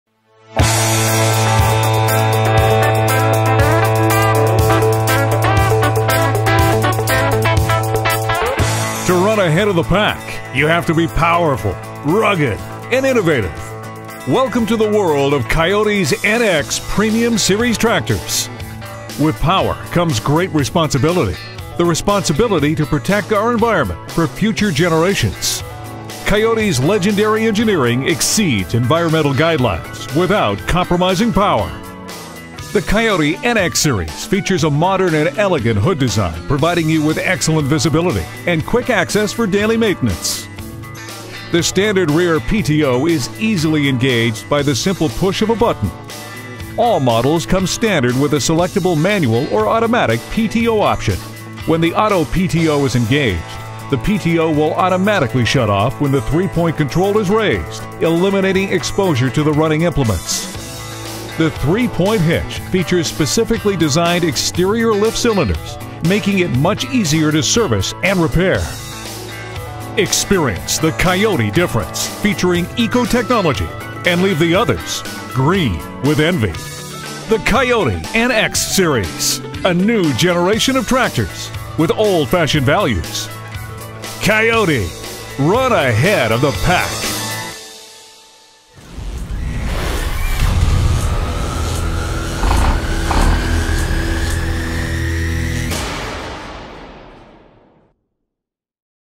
KIOTI NX-EXTERIOR
Check out the Exterior Features of KIOTI's New Premium NX Tractor Series.